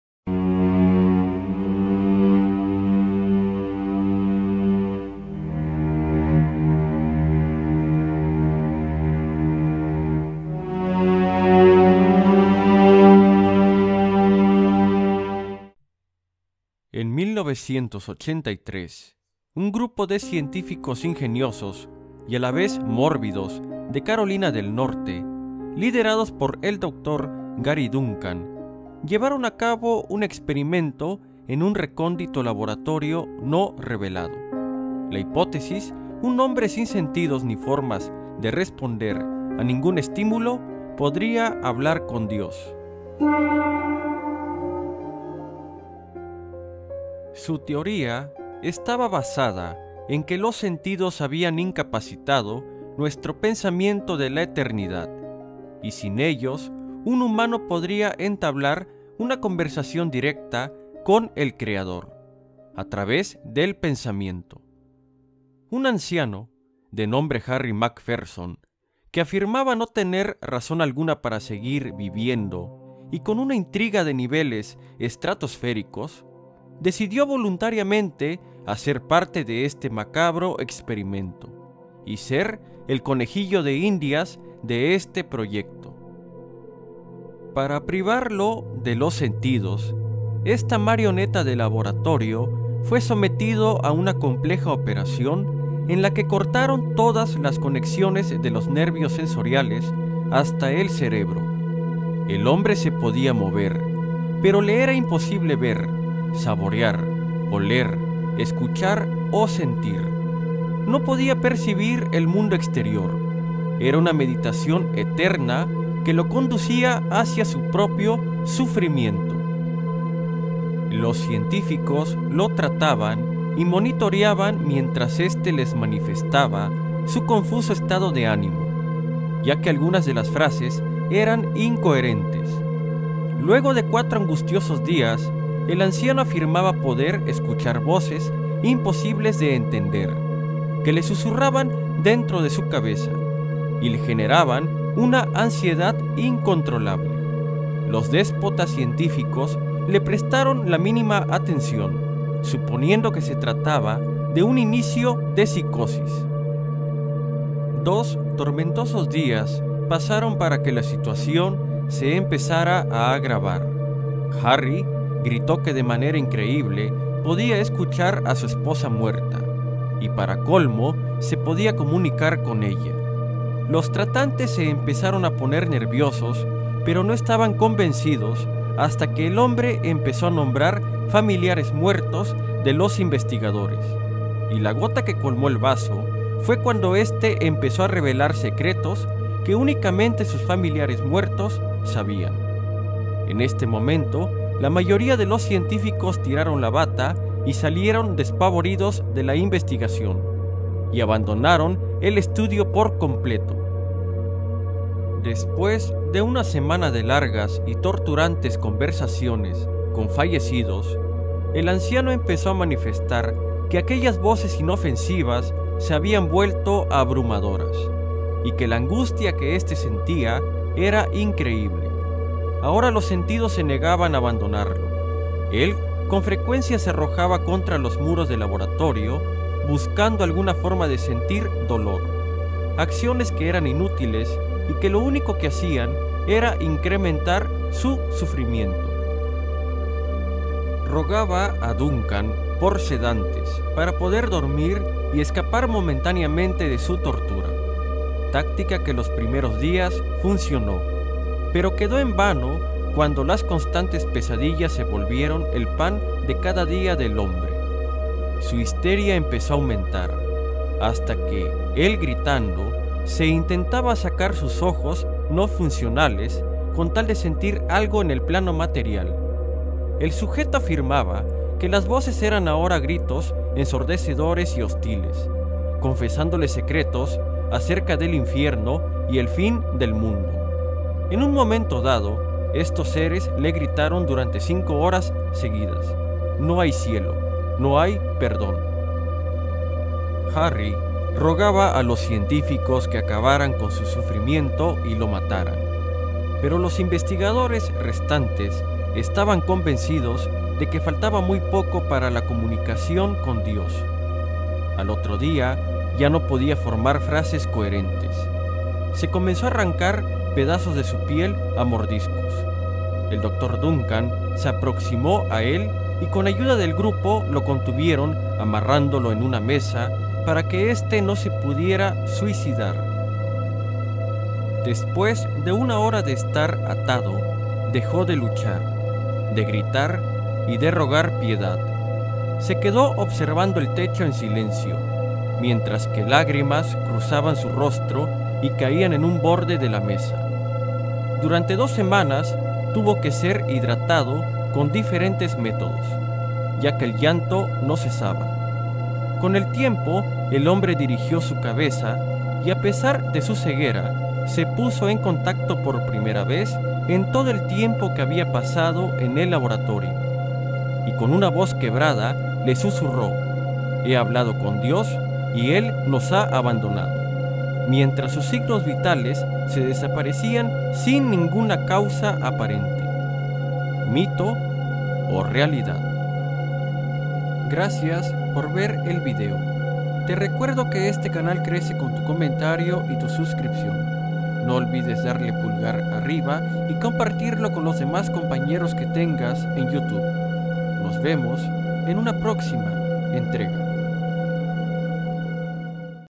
Un experimento aterrador | La historia de Harry McPherson
Un increíble y aterrador experimento mental.
¿Existe realmente un DIOS?.

¡Muchas gracias!